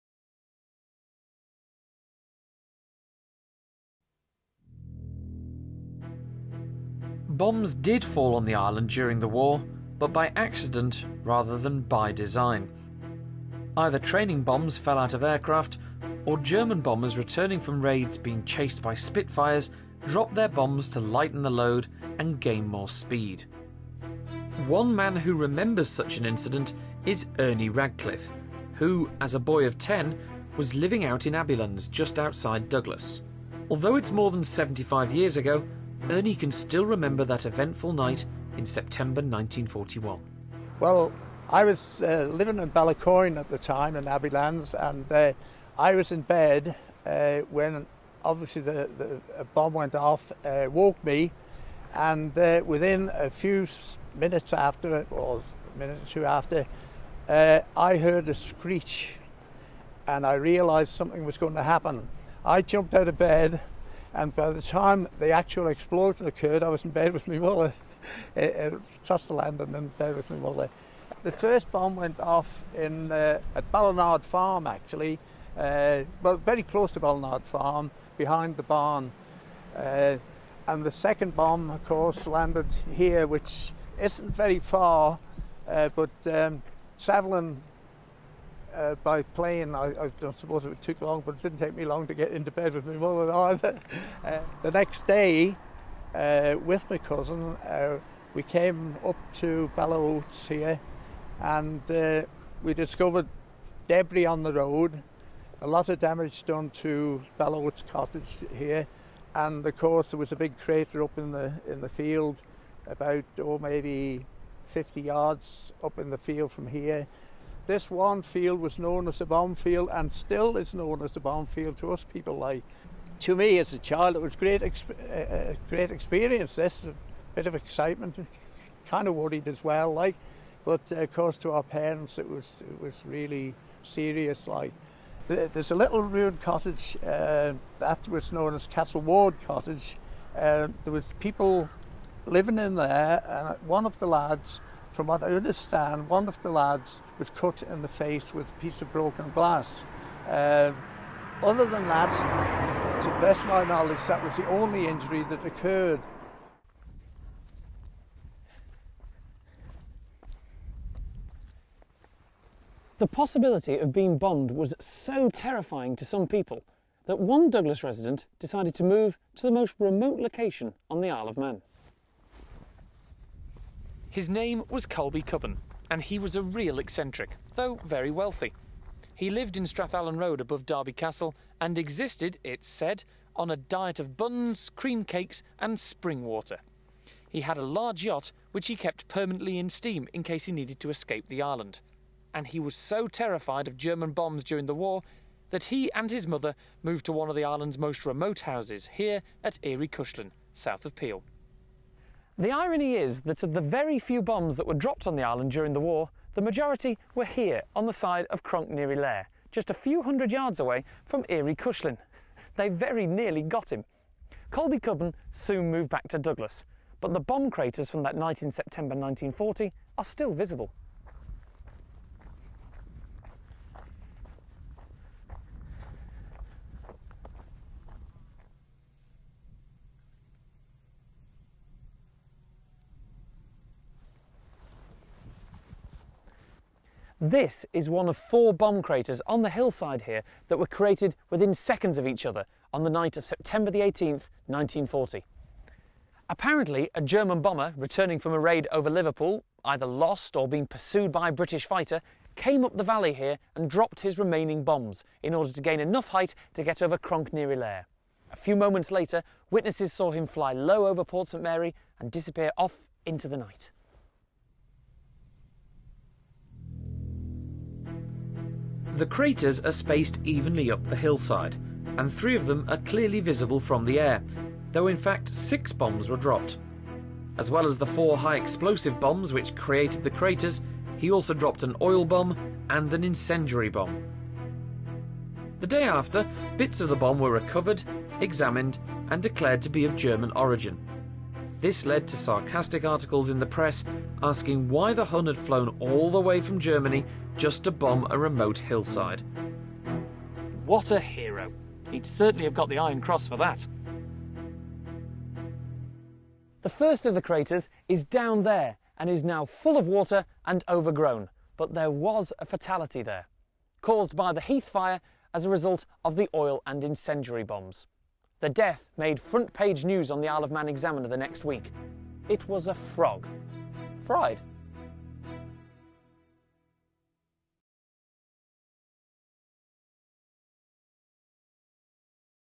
World War II Bombs in the Isle of Man: The Second World War in the Isle of Man, Pt. 9
The 9th part of a documentary about the Isle of Man during the Second World War.

Filmed by Culture Vannin.